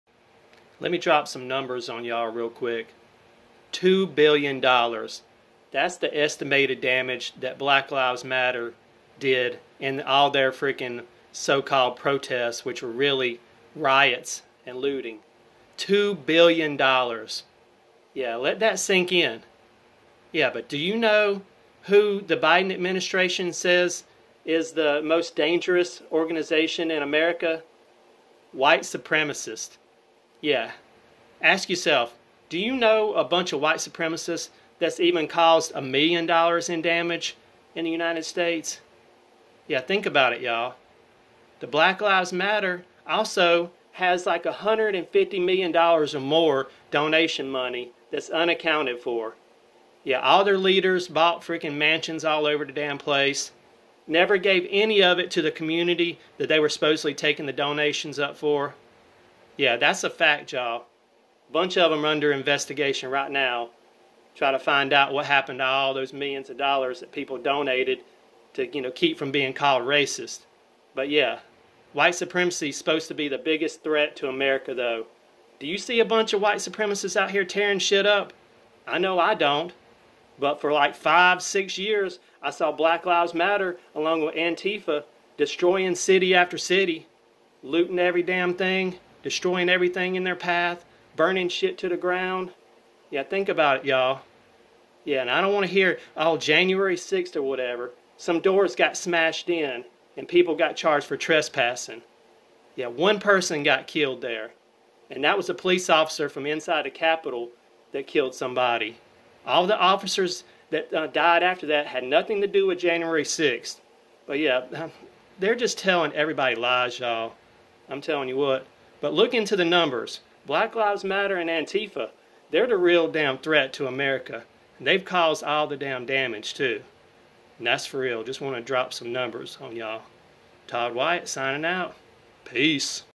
Dropping some #'s on y'all. The real terrorist in America.
2 billion dollars in estimated damages by BLM. F these entitled pieces of shee it. I do not care what you call me.